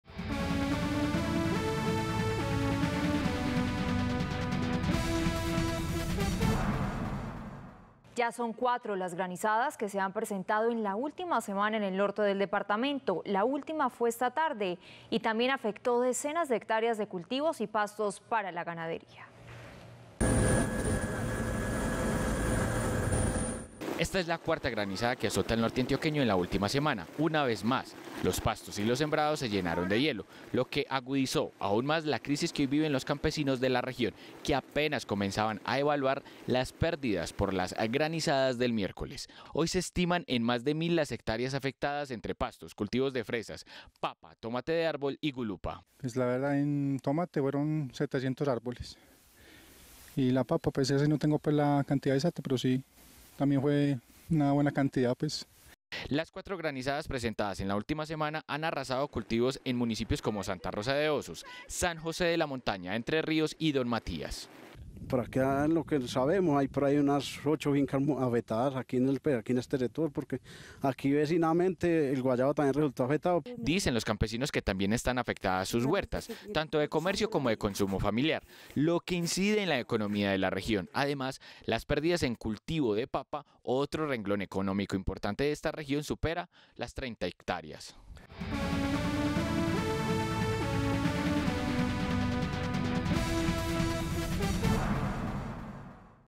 Otra granizada en el Norte de Antioquia - Teleantioquia Noticias
Ya son cuatro las granizadas que se han presentado en la última semana en el Norte de Antioquia. La última fue ayer en la tarde y afectó decenas de hectáreas de cultivos y pastos para la ganadería.